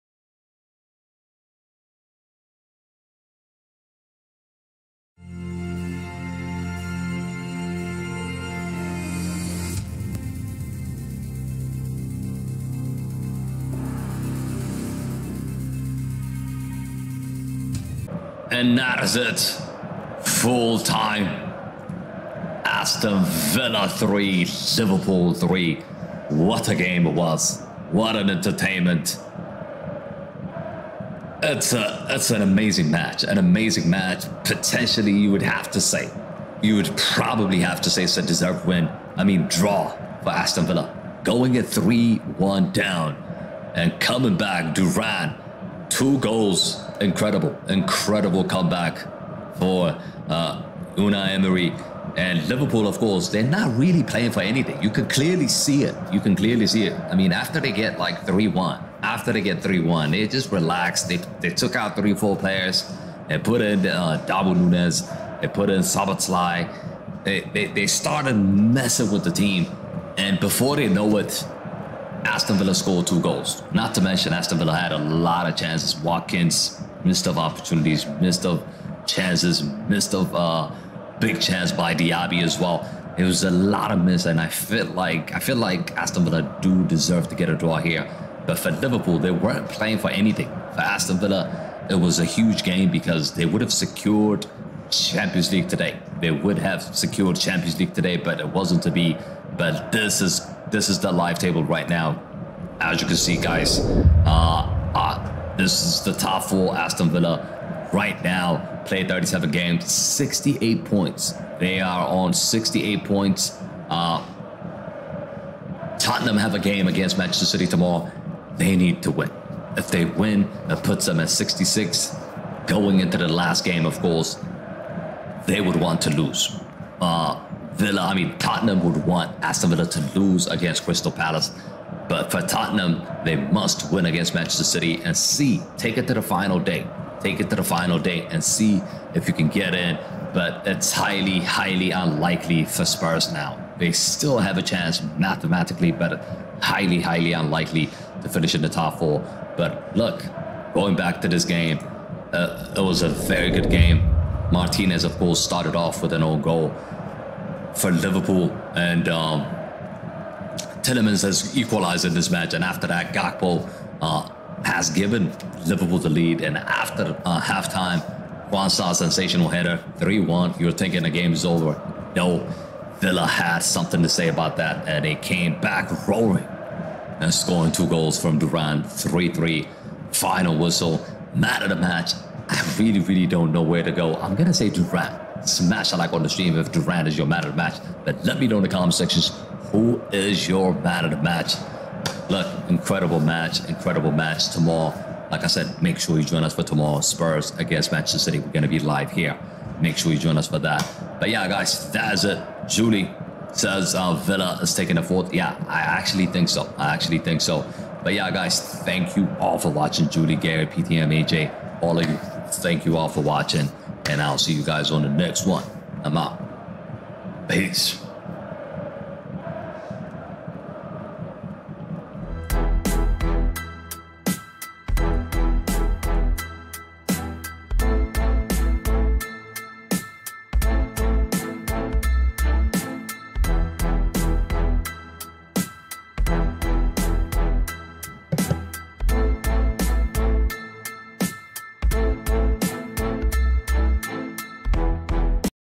DURAN DURAN! ASTON VILLA 3-3 LIVERPOOL
Aston Villa 3 Liverpool 3: Jhon Duran double seals dramatic late point for hosts in rollercoaster night for top 4 race!📱Download SofaScore, the best football app for stats, scores & more!